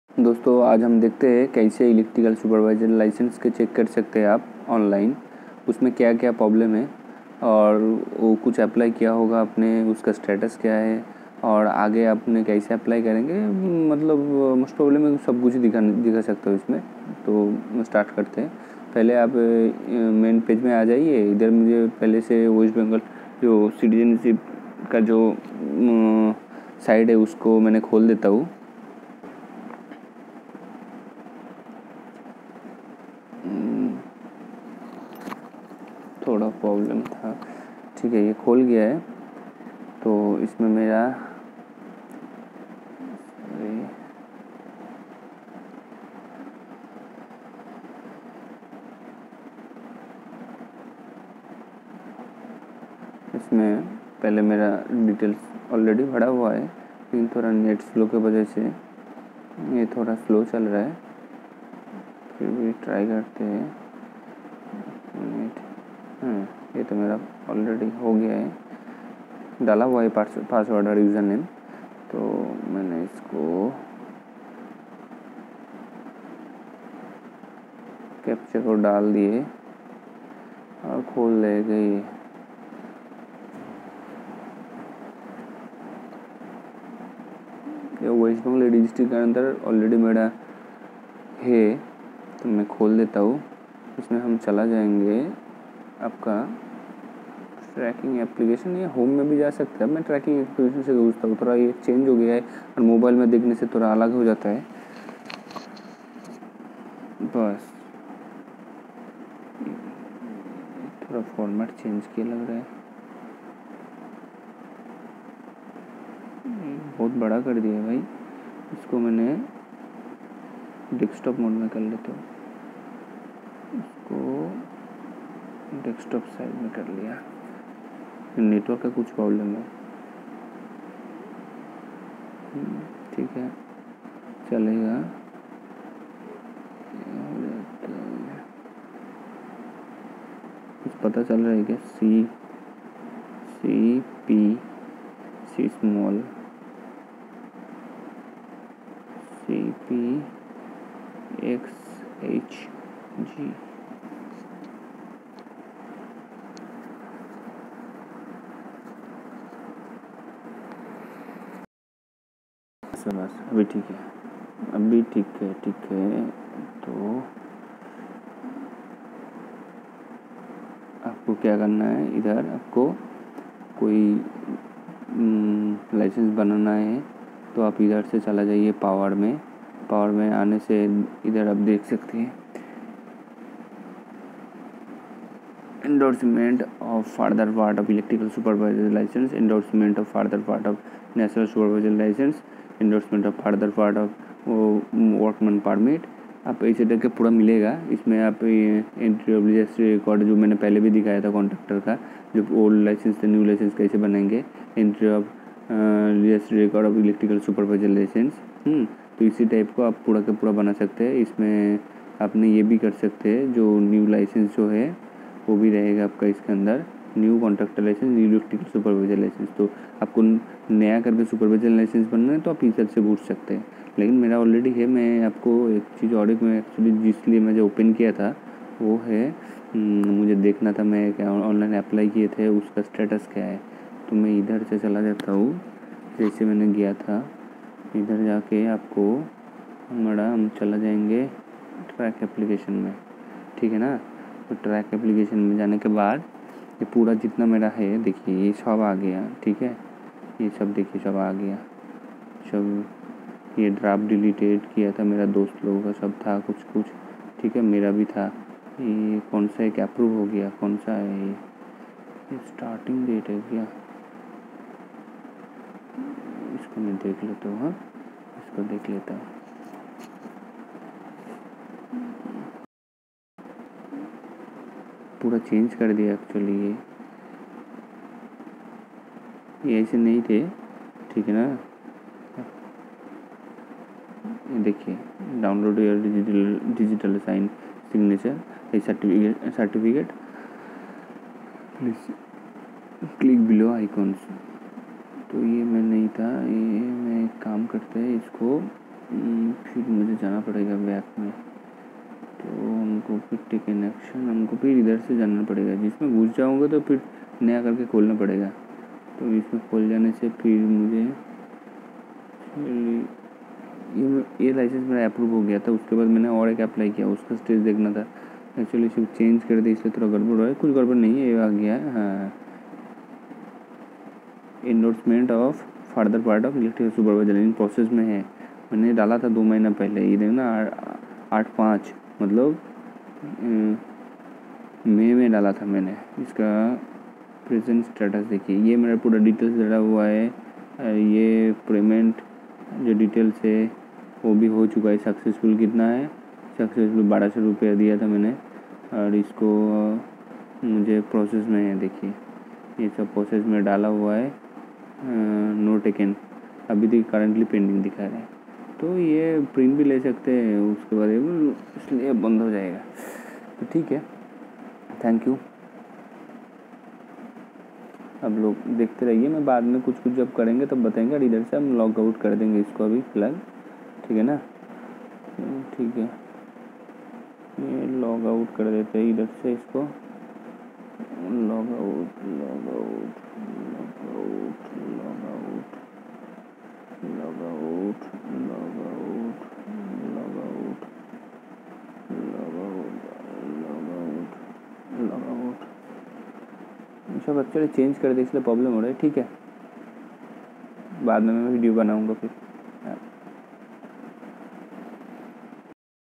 Check online your Electrical supervisor license wi
Hi I'm Dwipayan,
Today's video is electrical supervisor licence west bengal